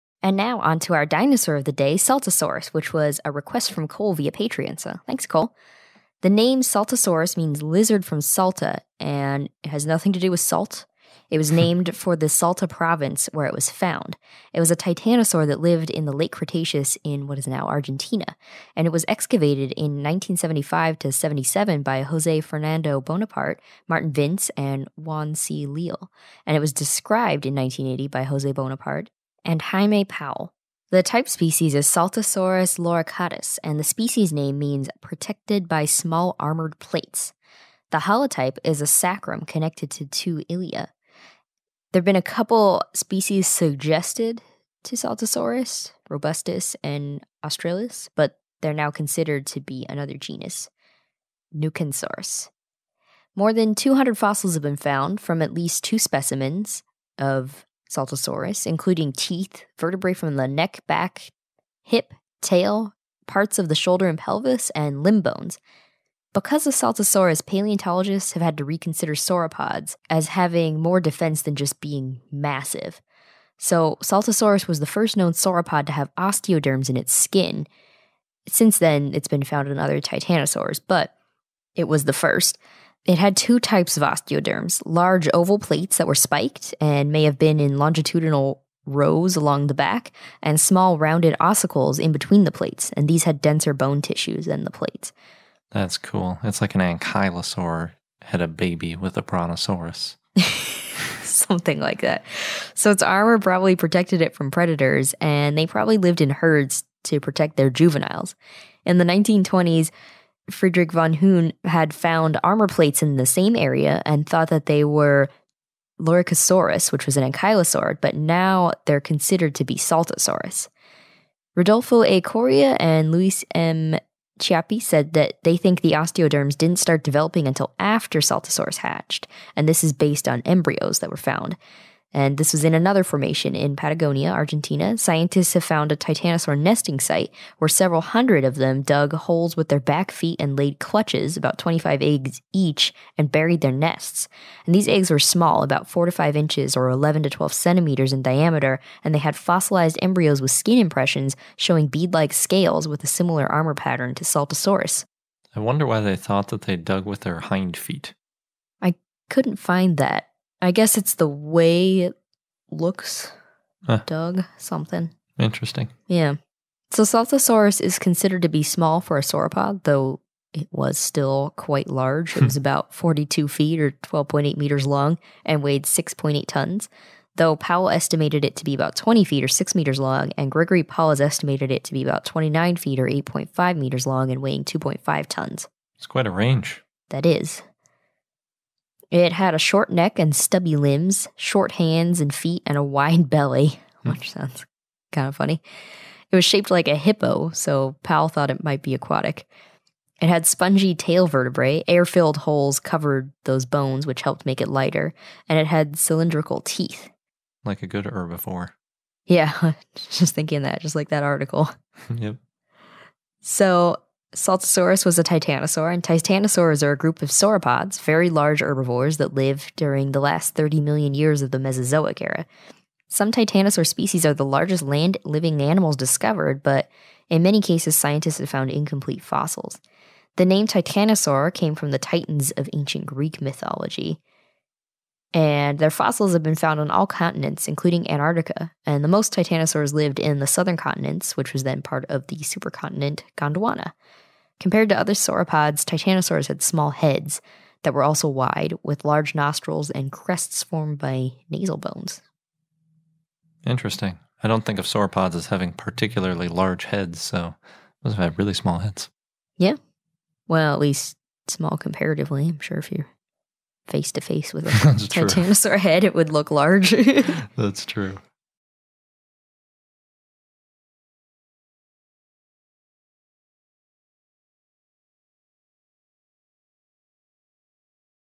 Saltasaurus: Dinosaur of the Day
Saltasaurus
Name means “lizard from Salta”, named for the Salta Province where it was found
Titanosaur that lived in the Late Cretaceous in Argentina
Excavated in 1975-77 by Jose Fernando Bonaparte, Martin Vince, and Juan C. Leal
Described in 1980 by Jose Bonaparte and Jaime Powell
Type species is Saltasaurus loricatus
Species name means “protected by small armored plates”
Holotype is a sacrum connected to two ilia
Saltasaurus robustus and Saltasaurus australis were suggested species, but are now cosidered to be Neuquensaurus
More than 200 fossils have been found, from at least 2 specimens, including teeth, vertebrae from the neck, back, hip, tail, parts of the shoulder and pelvis, and limb bones
Because of Saltasaurus, paleontologists had to reconsider sauropods (had more defense than just being massive)
First known sauropod to have osteoderms in its skin (since found in other titanosaurs)
Had two types of osteoderms: large oval plates that were spiked and may have been in longitudinal rows along the back, and small rounded ossicles in between the plates (these had denser bone tissue than the plates)
Armor probably protected it from predators, and they probably lived in herds, to protect juveniles
In the 1920s Friedrich von Huene had found armor plates in the area and thought they were Loricosaurus (ankylosaurid), but now they’re considered to be Saltasaurus
Rodolfo A. Coria and Luis M. Chiappe said they think the osteoderms didn’t start developing until after they hatched (based on the embryos found)
In another formation, in Patagonia, Argentina, scientists have found a titanosaur nesting site, where several hundred of them dug holes with their back feet and laid clutches, about 25 eggs each, and buried their nests. Eggs were small, about 4-5 in (11-12 cm) in diameter, and had fossilized embryos with skin impressions showing bead-like scales, with a similar armor pattern to Saltasaurus
Considered to be small for a sauropod, though it was still large
About 42 ft (12.8 m) long and weighed 6.8 tons, though Powell estimatesd it to be about 20 ft (6 m) long and Gregory Paul estimated it to be about 29 ft (8.5 m) long and weighing 2.5 tons
Had a short neck, stubby limbs
Had short hands and feet and a wide belly
Shaped like a hippo, so Powell thought it was aquatic
Had spongy tail vertebrae (air-filled holes covered those bones, which helped make it lighter)
Had cylindrical teeth